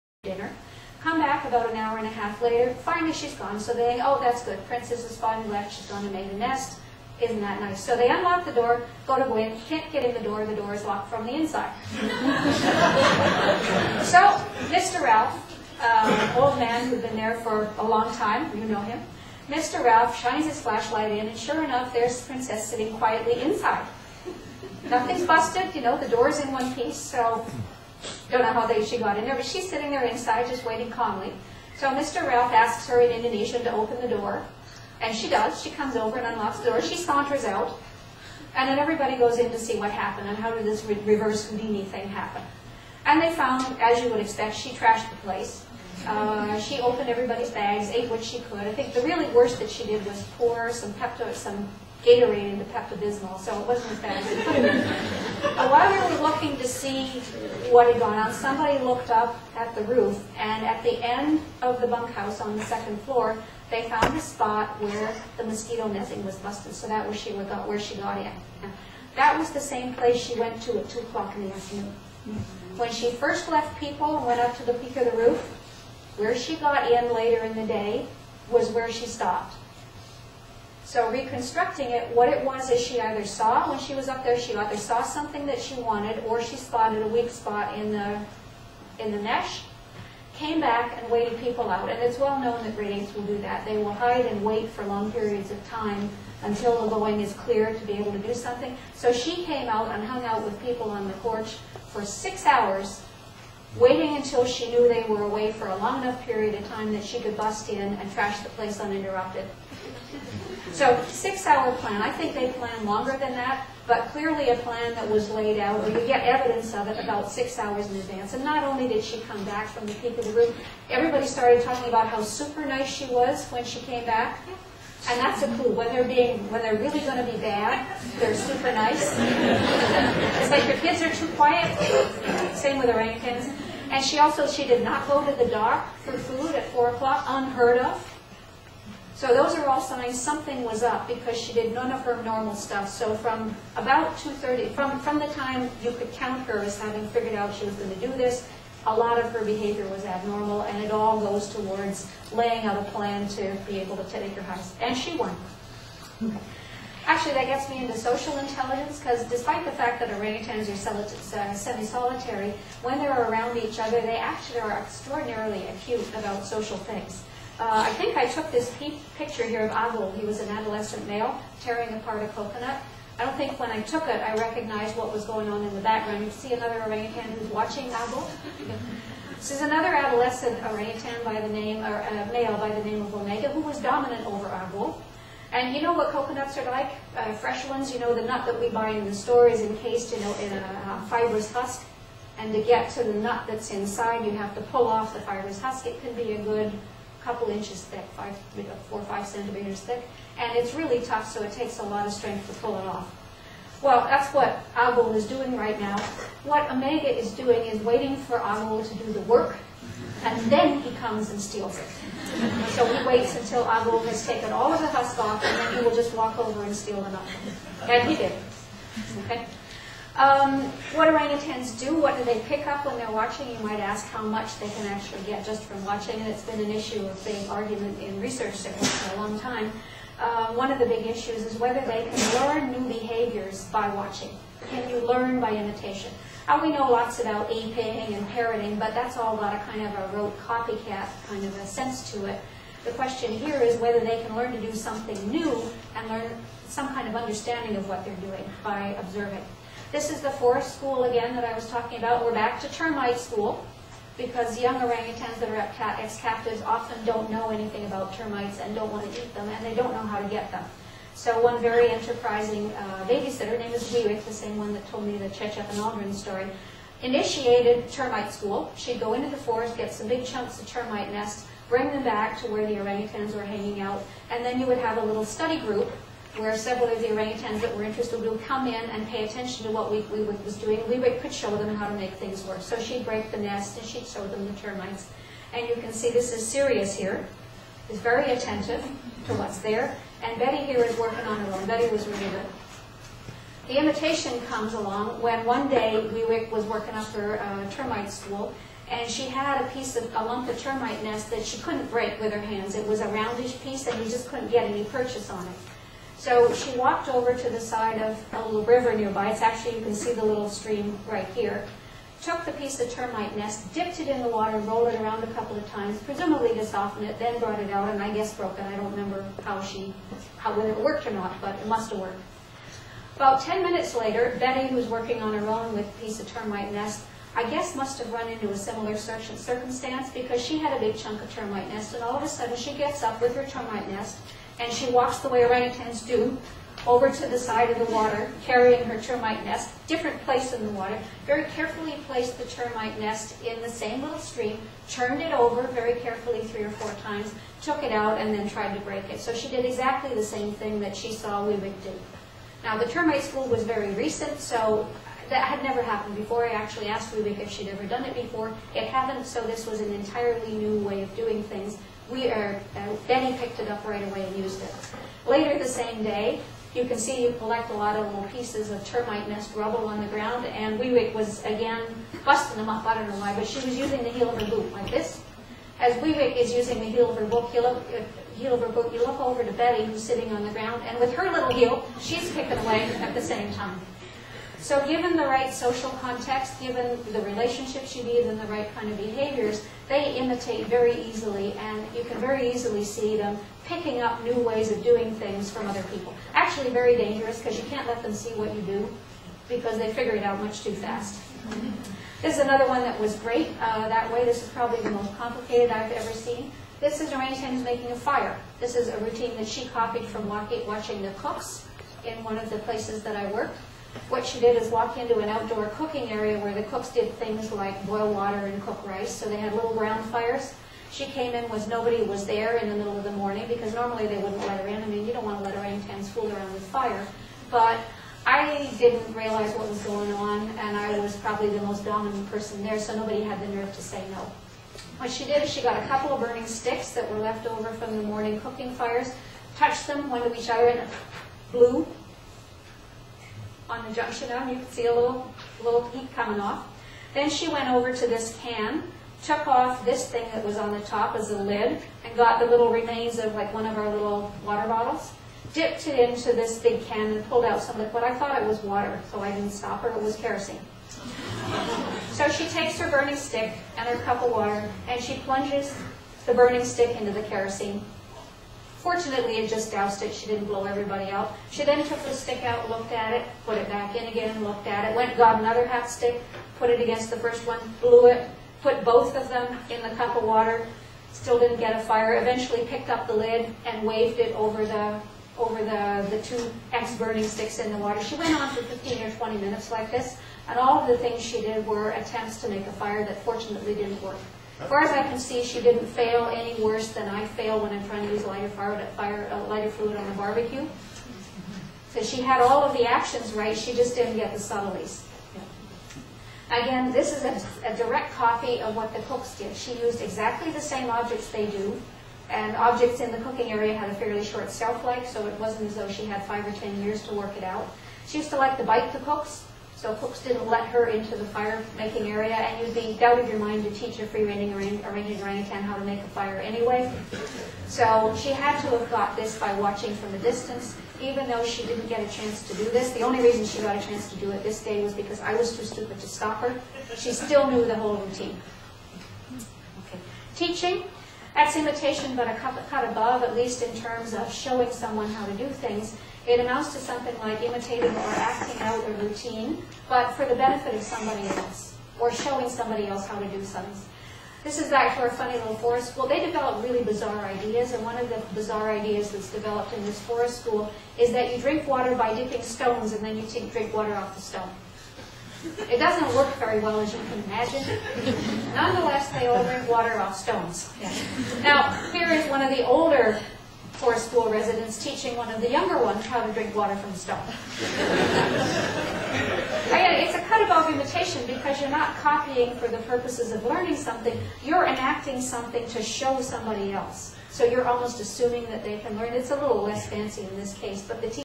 Close Encounters with the Red Ape 5 of 8
BOS Canada presents:
A lecture and fundraiser
All proceeds to orangutan conservation

CELEBRATING 20 YEARS WITH ORANGUTANS IN BORNEO
Dr. Anne Russon
renowned author & primatologist

Orangutan Island scientific advisor, Quirks & Quarks
guest, Orangutans-wizards of the rainforest author.